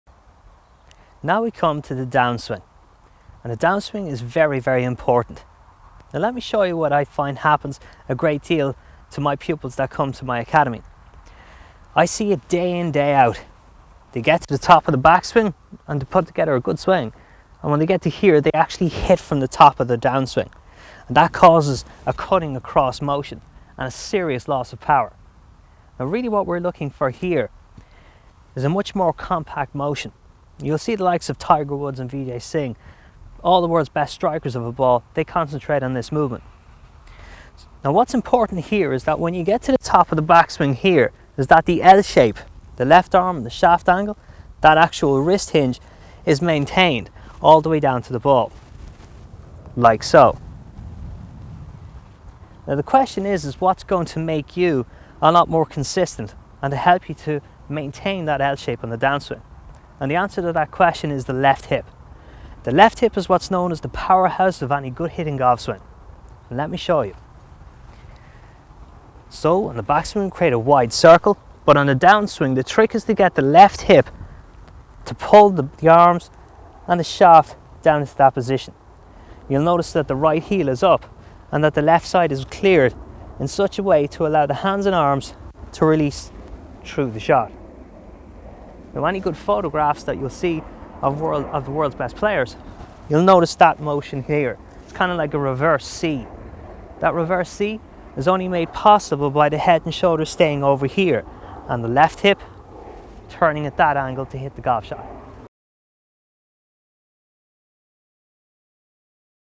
How to create a great down swing for awesome power.
is the home website of Ireland's No1 golf lesson instructor Barry Power, in this video barry shares his secrets on to create a great downswing.